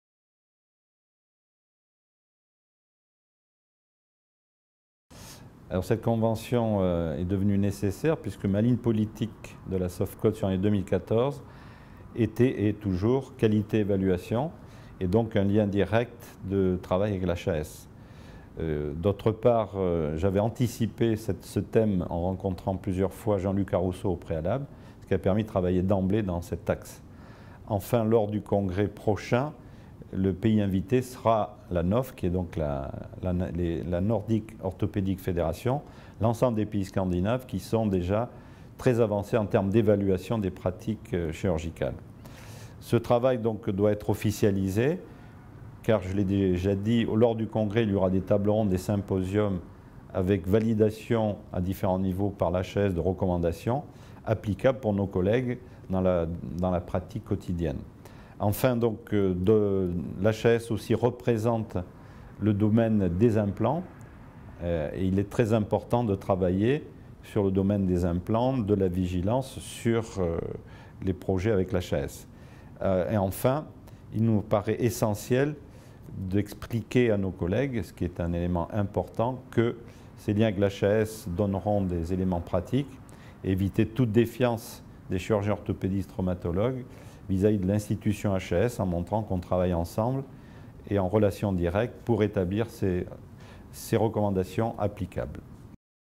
HAS - Pourquoi avoir souhaité signer une convention avec la HAS ?
Pr Norbert Passuti, président du CNP-Sofcot.

Pour consulter le dossier spécial "Orthopédie & Traumatologie" de la Haute autorité de santé